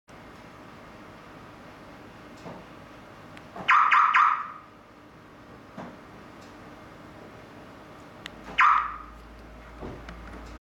ハリウッド映画に出てくるドアロック音 LOCK音Ver.2.7 メルセデス･ベンツAMG C63S
ハリウッド映画に出てくるドアロック音
LOCK音Ver.2.7 MONO TONE
(LOCK音サウンドアンサーバックキット)
■装着車両：メルセデス･ベンツ AMG C63S
サウンド設定
・SOUND　 ：F
・INTERVAL：6
※メルセデス･ベンツ標準ハザードアンサーバックに合わせて施錠時3回、解錠時1回鳴る設定にしています。